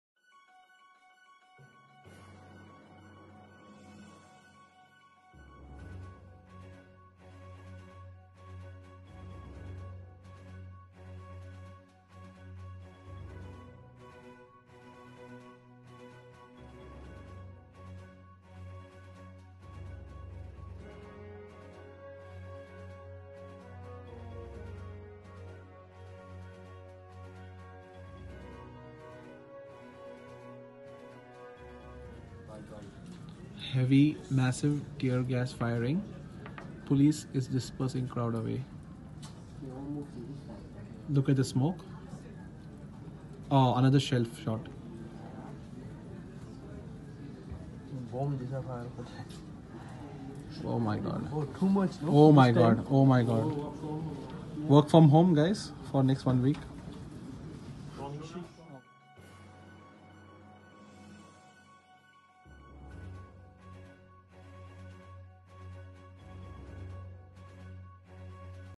Hong Kong protests against extradition bill
Hong Kong protested extradition bill in a big way. one millions people have gathered over last two days. check out video for more details.